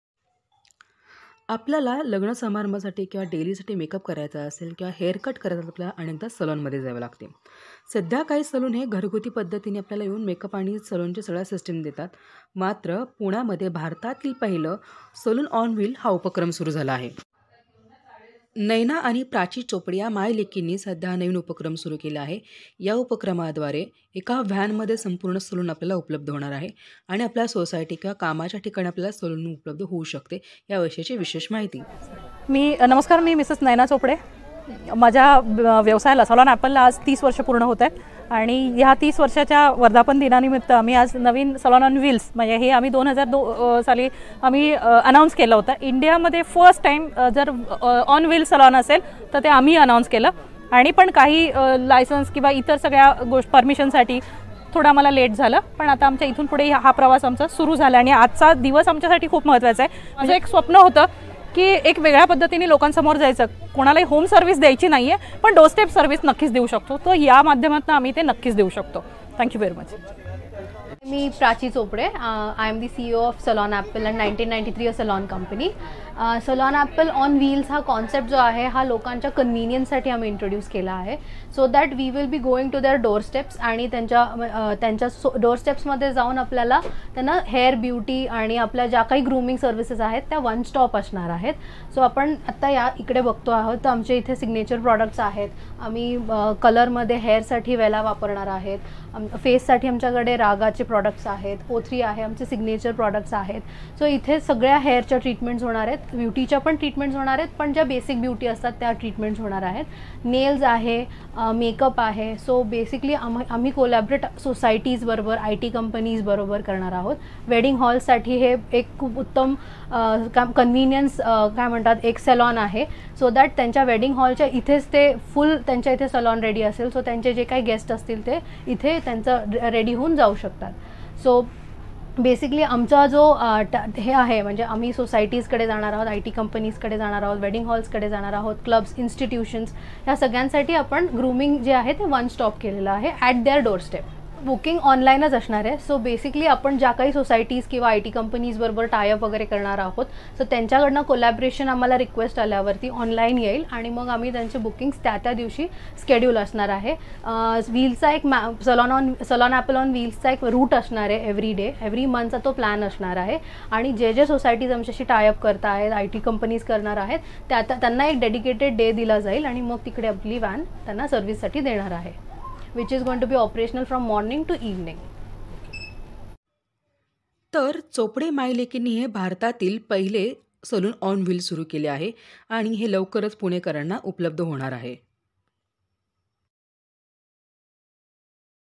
Success Story | पुण्यातील मायलेकींनी सुरु केलं चालतं फिरतं सलून, आता तुमच्या घरीच येणार सलून व्हॅन
Success Story: पुण्यातील सलॉन ॲपलचा 30 वा वर्धापन दिन नुकताच साजरा झाला. त्यानिमित्तानं नयना आणि कांचन चोपडे या मायलेकींनी 'सलून ऑन व्हिल' ही सर्व्हिस सुरू केली आहे. यामध्ये तुमच्या दारातच सलूनची व्हॅन येणार असून एकाच व्हॅनमध्ये वेगवेगळ्या प्रकारचा मेकअप करता येईल.